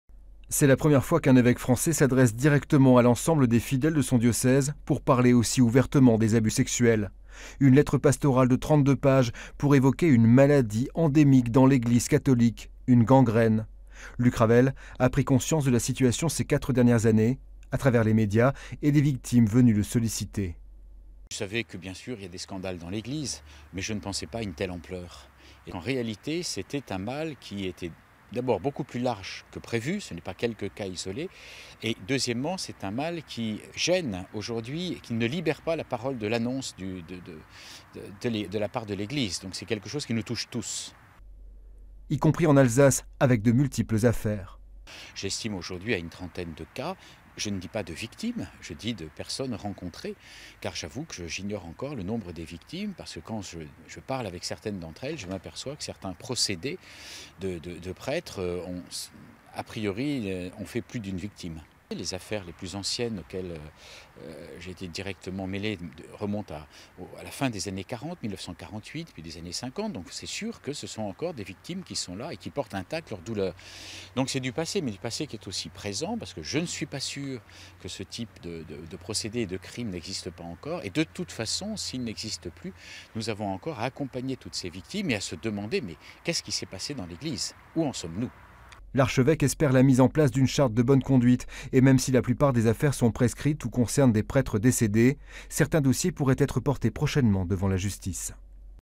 Monseigneur Ravel brise l'omerta sur les abus sexuels dans l'Eglise
Monseigneur Ravel, l'Archevêque de Strasbourg publie une lettre pastorale pour dénoncer les abus sexuels dans l'Eglise de France. Le reportage

RÉDIGER UN CHAPÔ